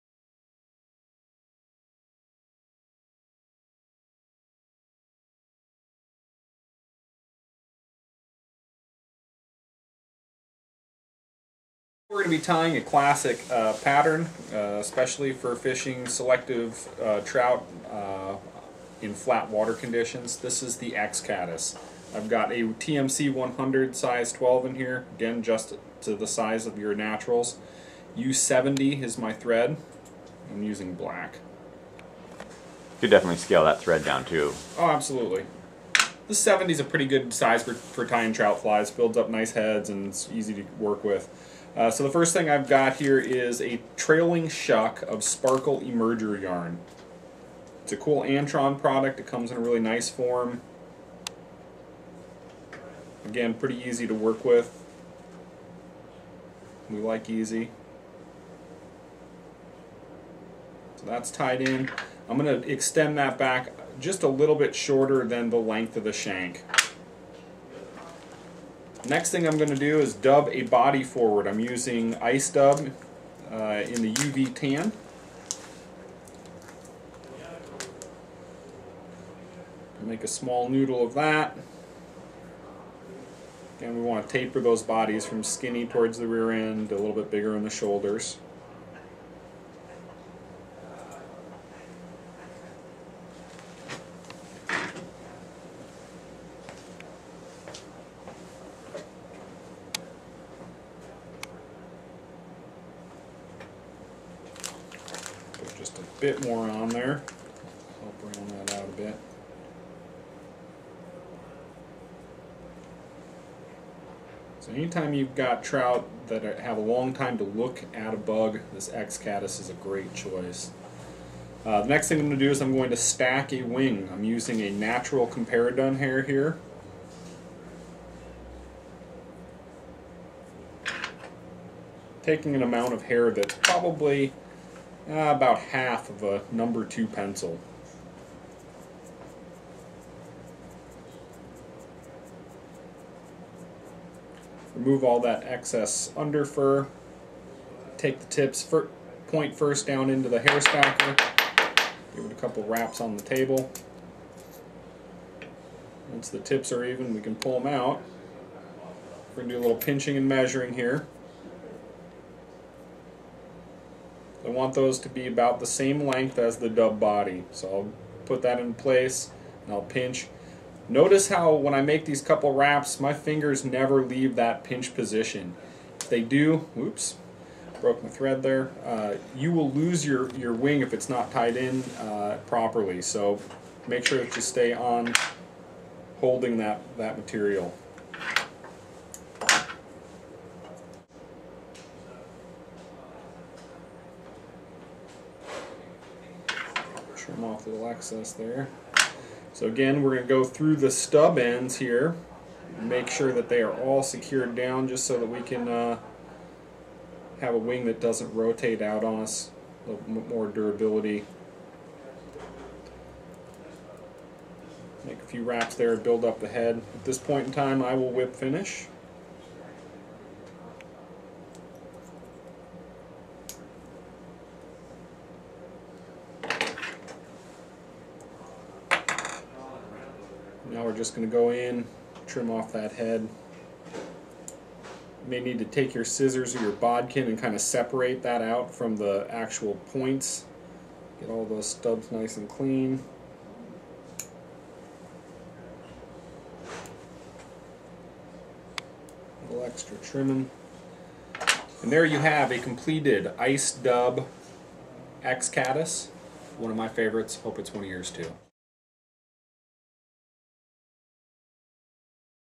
Ice Dub X-Caddis Fly Tying Video
UV treated fly tying materials are one of the hottest trends in fly tying and fishing. In this short instructional video Barrett Christiansen of Caddisflyshop.com demonstrates how to tie an X- Caddis using UV Ice Dub. UV Ice Dub is currently the number one selling dubbing on the planet.  The x-caddis is one of our all time favorite and effective Caddis Patterns